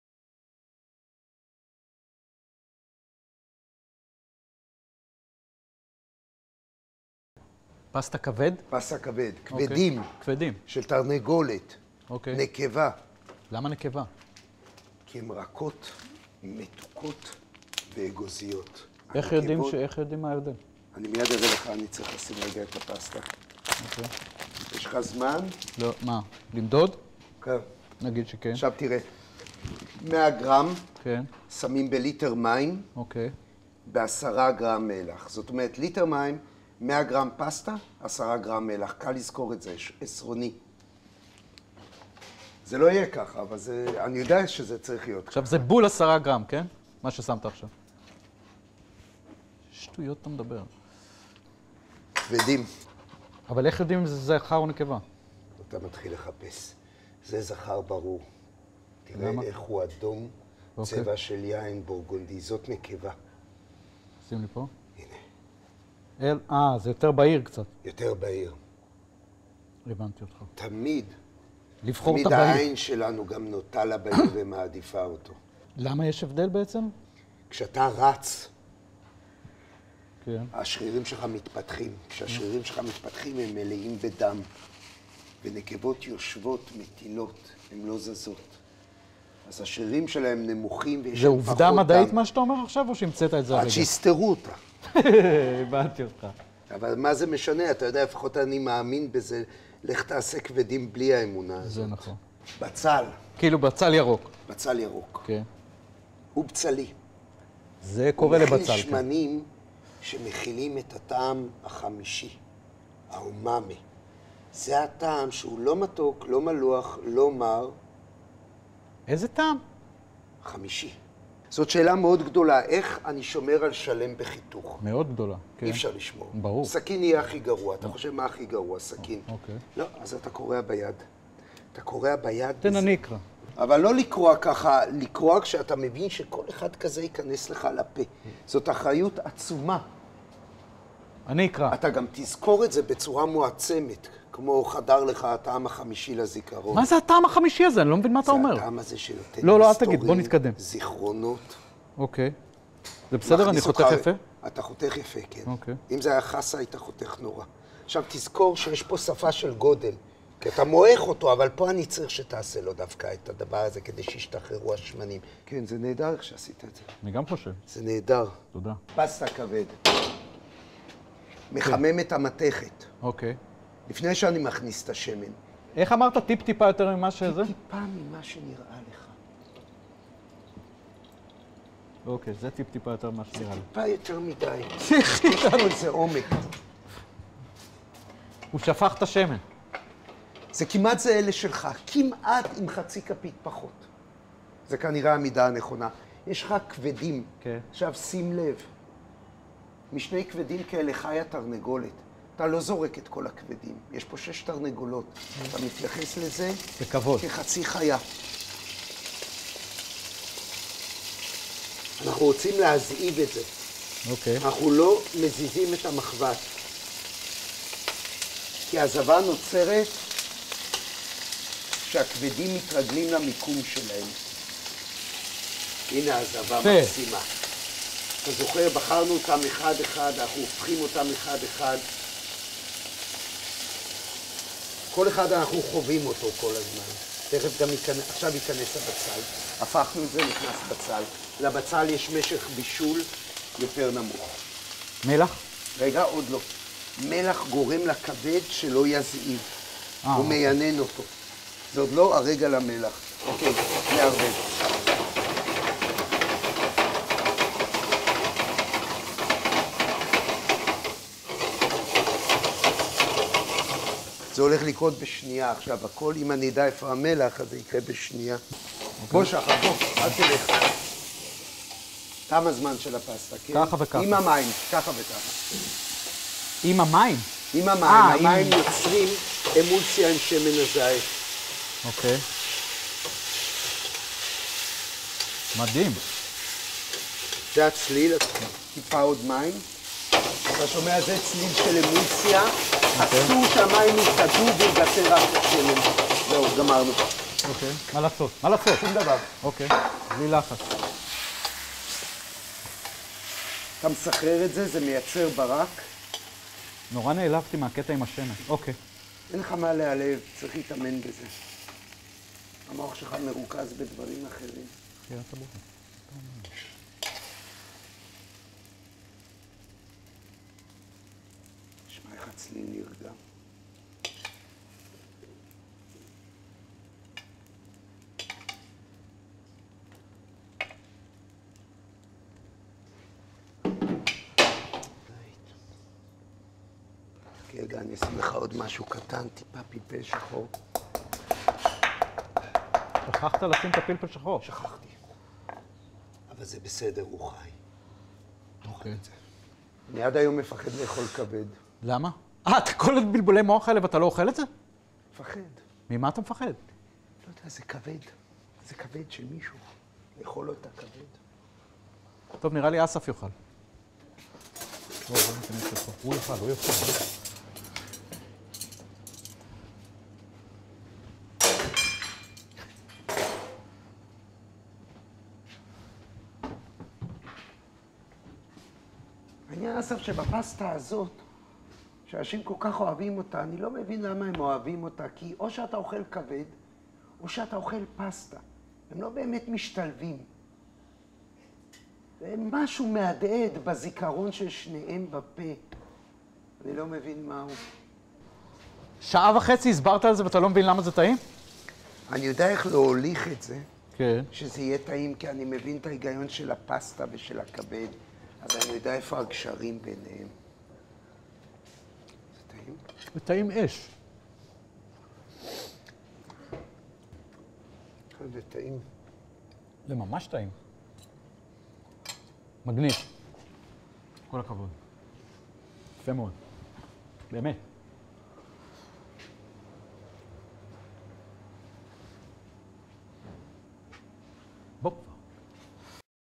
Eyal Shani & Shahar Segal Ep.2: Chicken Liver Pasta
Director: Gil Levanon
Editor : Shai Appelberg
Production : POV
Chef: Eyal Shany & Shahar Segal